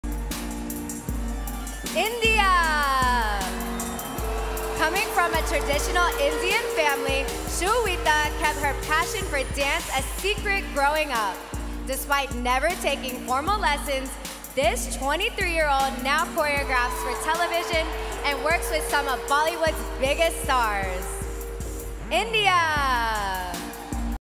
Blaze of brilliance: Shweta Sharda stuns in swimwear, setting ablaze the stage at Miss Universe 2023
Behold, ladies and gentlemen! Prepare to be enchanted as we unveil a spectacle of sheer brilliance. Infusing an extra dose of femininity and gentleness, the stunning lady makes an entrance at the 72nd Miss Universe Preliminary Competition. Each stride, a celebration of curves, captivating the audience with an enthralling walk that sets hearts ablaze! Unravelling a tapestry of unparalleled allure and refined elegance, the queen dazzles in her swimwear, foretelling a triumphant journey to seize the coveted crown of Miss Universe 2023. Immerse yourselves in the complete video and witness our queen gracefully claim it all in one grand sweep!